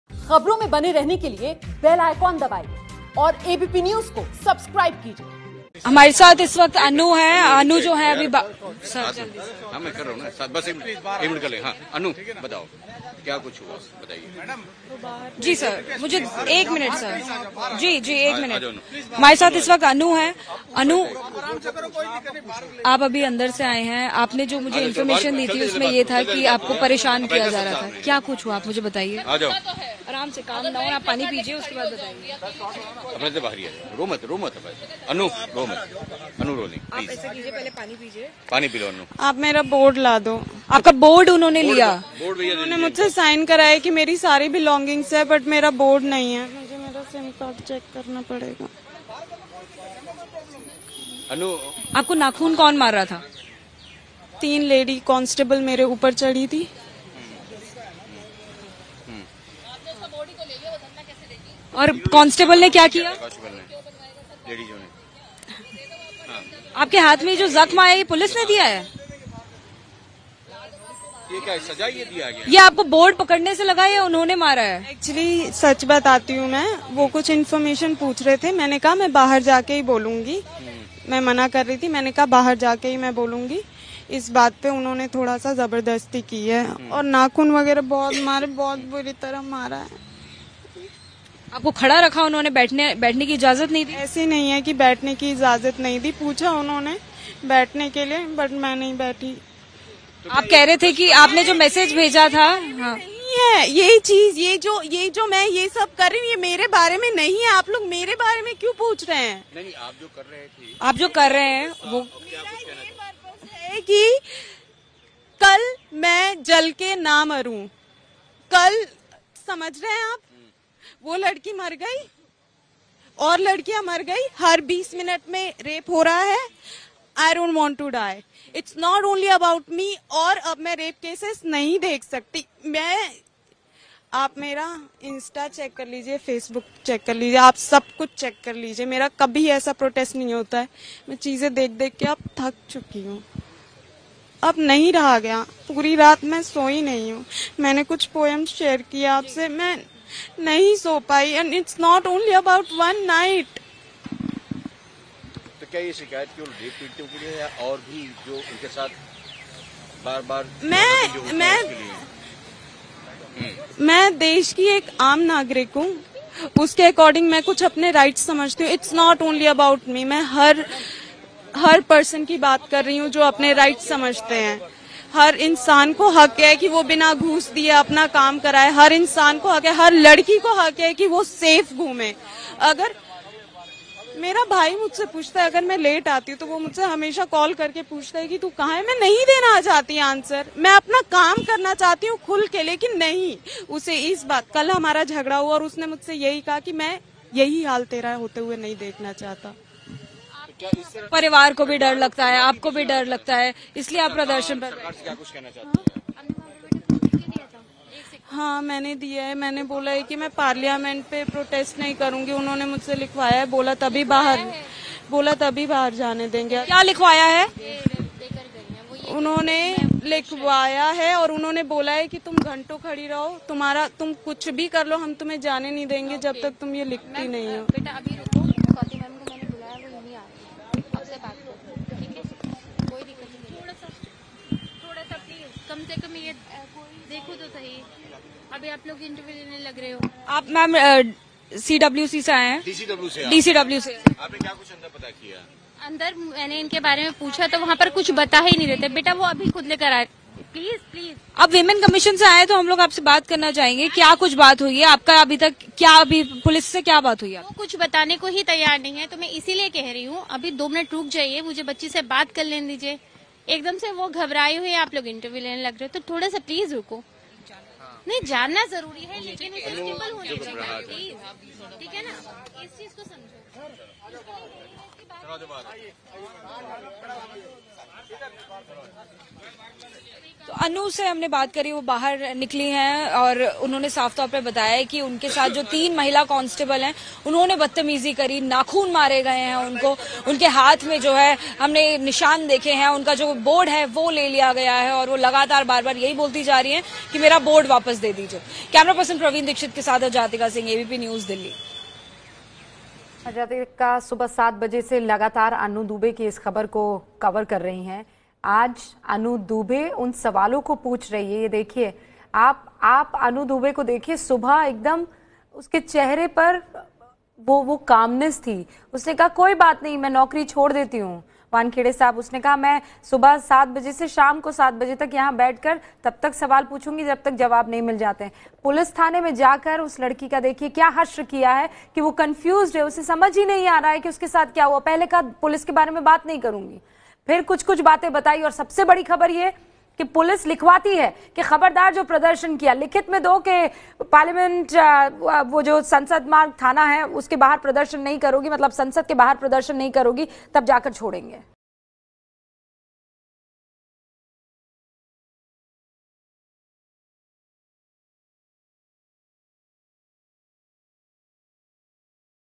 दिल्ली पुलिस की बर्बरता से सहमी Anu Dubey, कहा- 'बहुत बुरी तरह से मारा' | ABP News Hindi
दिल्ली पुलिस का बर्बर चेहरा सामने आया है. दिल्ली पुलिस ने अनु को बुरी तरह से मारा. अनु ने बताया कि तीन लेडी कांस्टेबल उनके ऊपर चढ़ी हई थी. उन्हें बुरी तरह से नाखून मारे गए. ये सब बताते हुए अनु की आंखें छलक गई. वो बार बार ये कह रही थीं कि उन्हें बुरी तरह से मारा गया है.